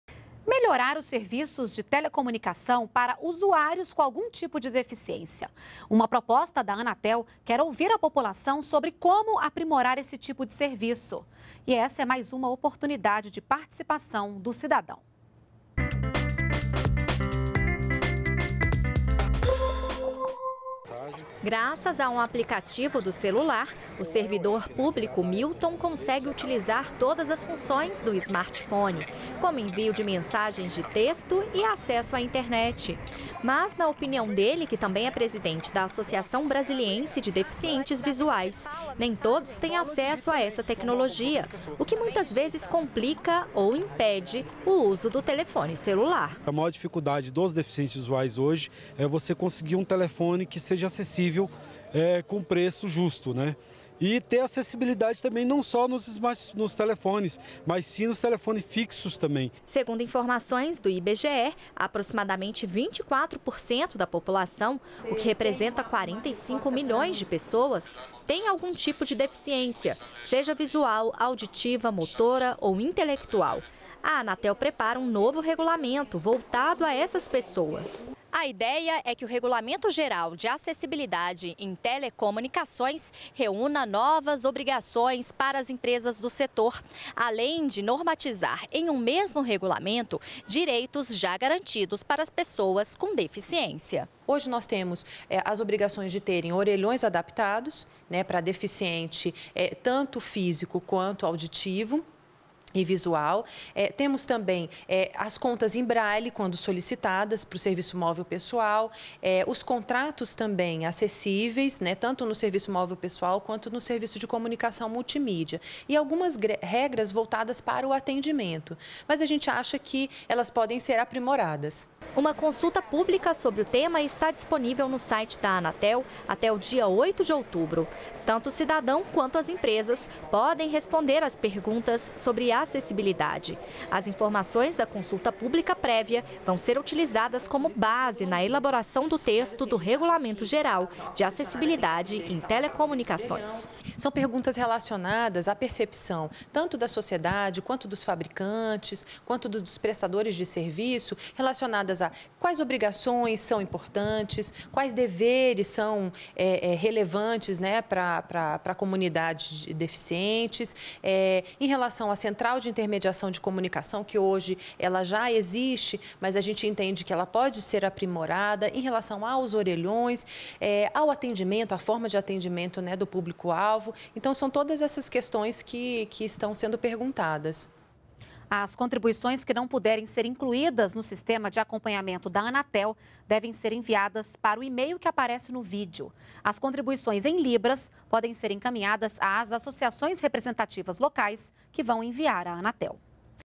Cidadãos podem dar sugestões sobre serviços de telecomunicação para usuários com deficiência
NBR NOTÍCIAS - 16.09.14: A Agência Nacional de Telecomunicação (Anatel) abriu consulta pública para a elaboração do Regulamento Geral de Acessibilidade em Telecomunicações, que irá reunir novas obrigações para as empresas do setor. O objetivo é melhorar os serviços de telecomunicação para usuários com deficiência que, segundo o IBGE, ocupam cerca de 24% da população brasileira. Os interessados em participar da consulta podem enviar sugestões até o dia 8 de outubro para o As contribuições que não puderem ser incluídas no Sistema de Acompanhamento da Entidade devem ser enviadas para o endereço: . As sugestões em libras podem ser encaminhadas às associações representativas locais, para envio à Anatel.